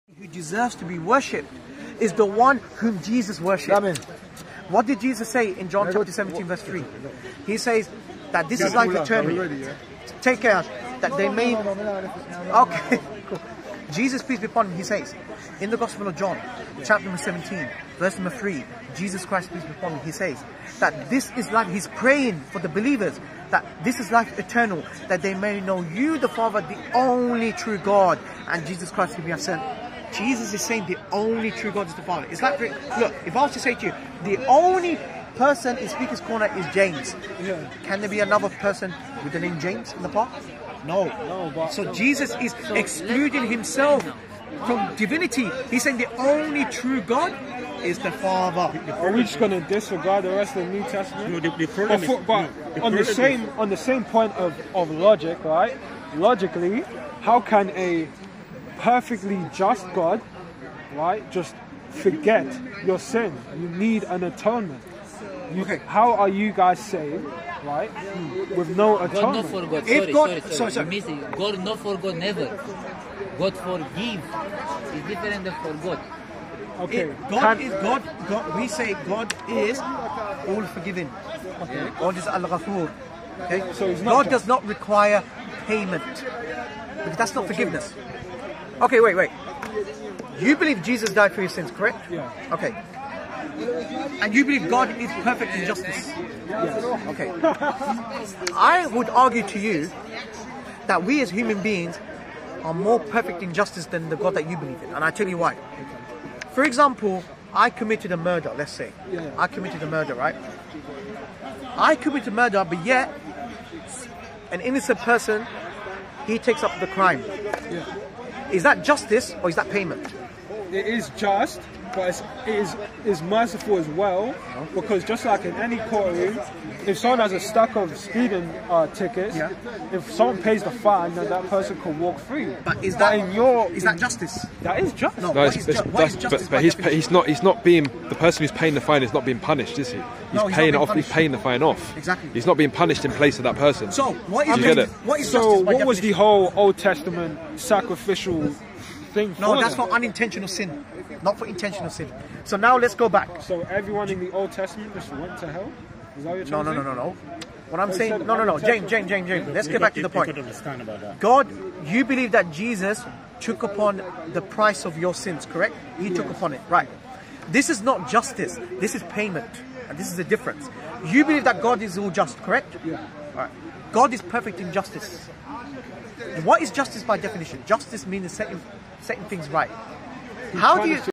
Is Jesus Dying For Others Justice? Rayhan & Christian @ Speaker's Corner
Is a man being executed for the sins of others fair?